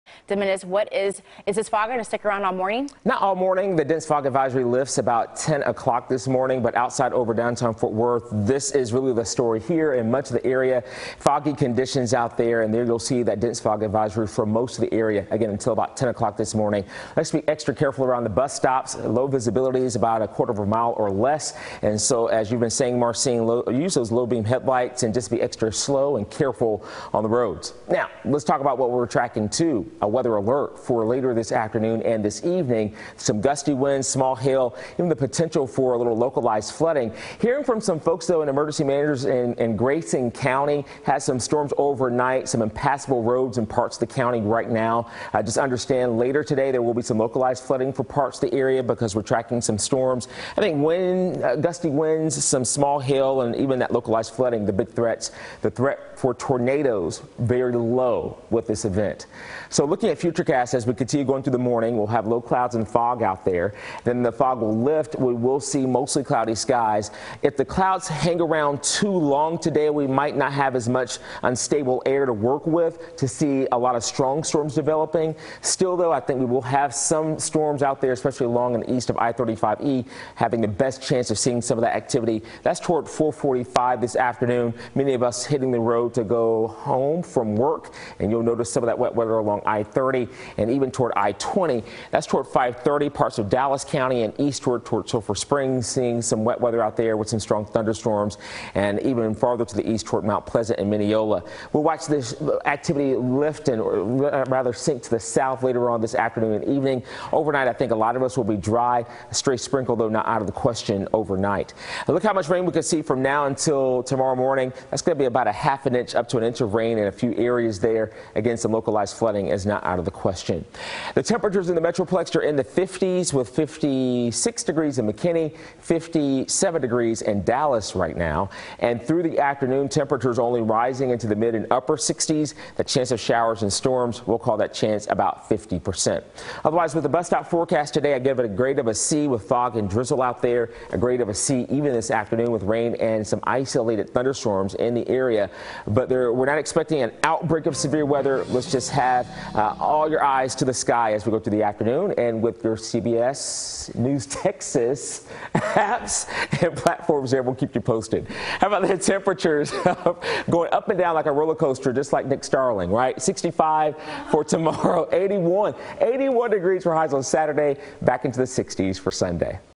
Roller coaster weather ahead for North Texas this weekend
Our temperature roller coaster ride will continue into the weekend. In fact, highs will be in the low 80s Saturday, then they'll fall into the mid 60s on Sunday after a cold front passes by.